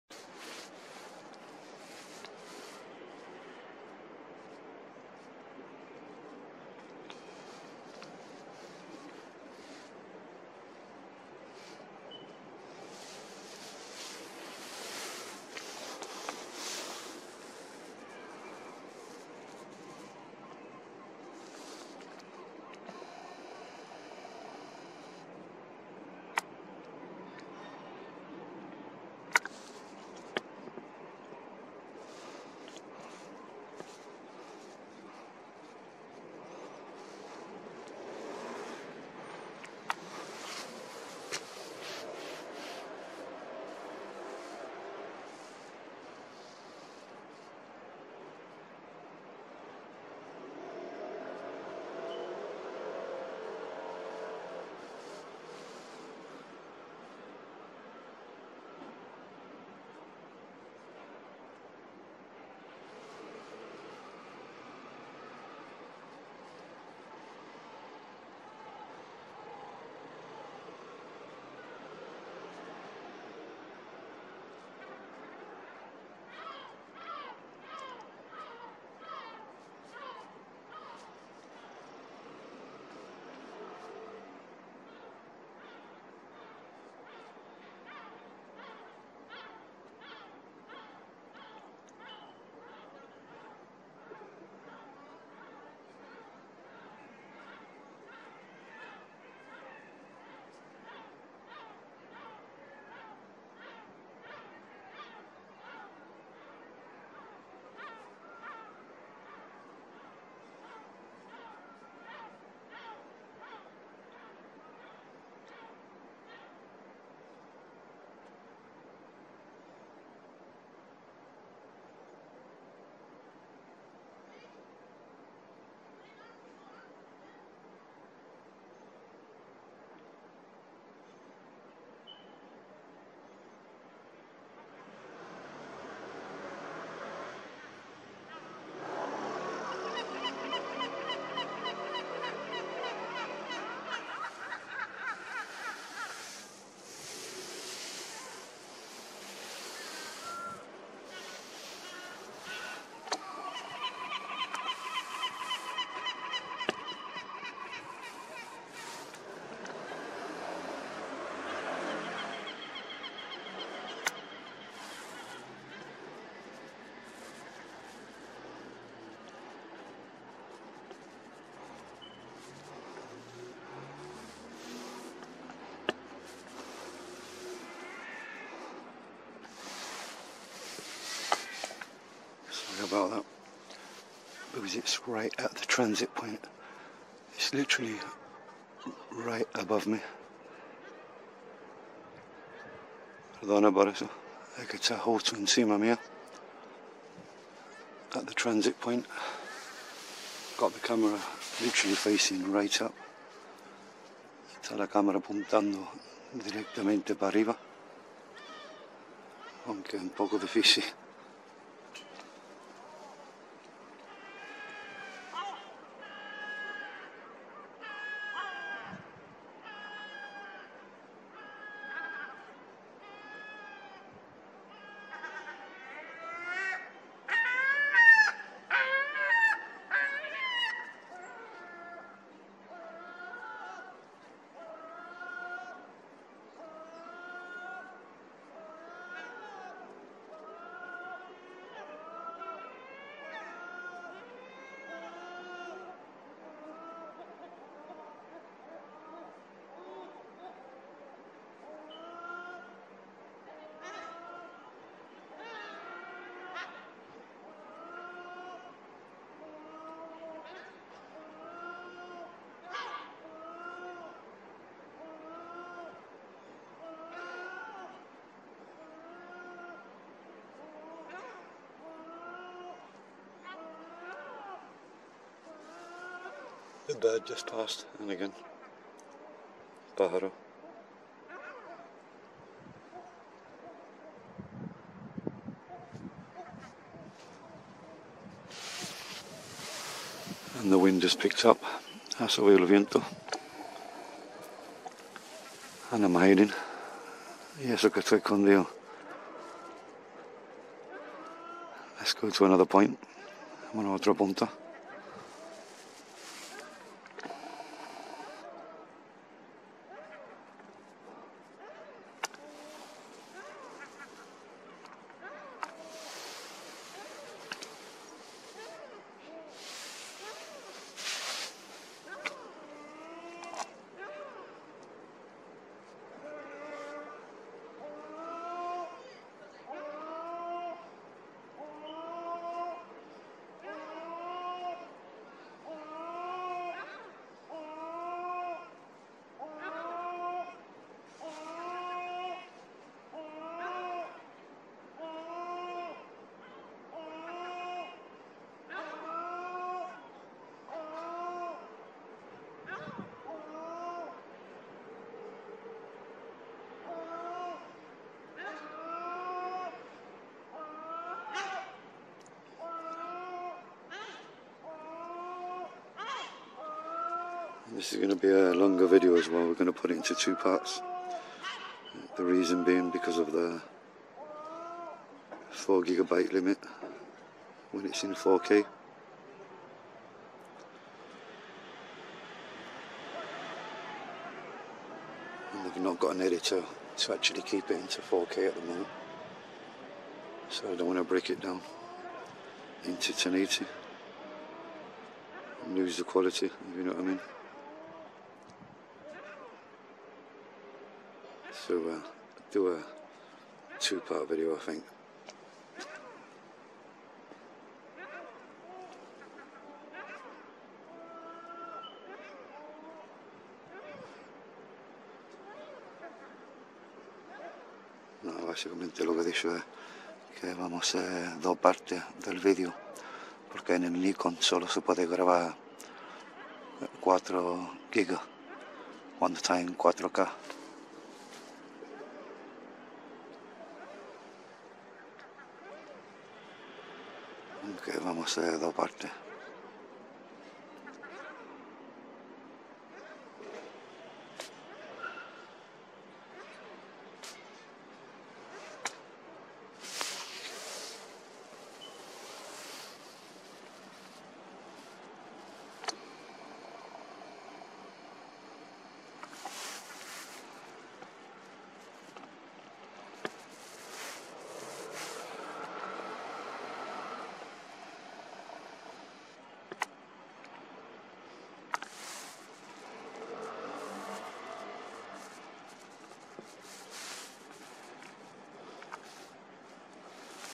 92.8% Moon / Luna - at it's Transit Point 22:30 Thursday 2nd February 2023 Gibraltar 🇬🇮 Med - Part 1
92.8% Moon / Luna - at it's Transit Point 22:30 Thursday 2nd February 2023 Gibraltar 🇬🇮 Med - Part 1 - Pic's on Facebook and TikTok Ledgib ✌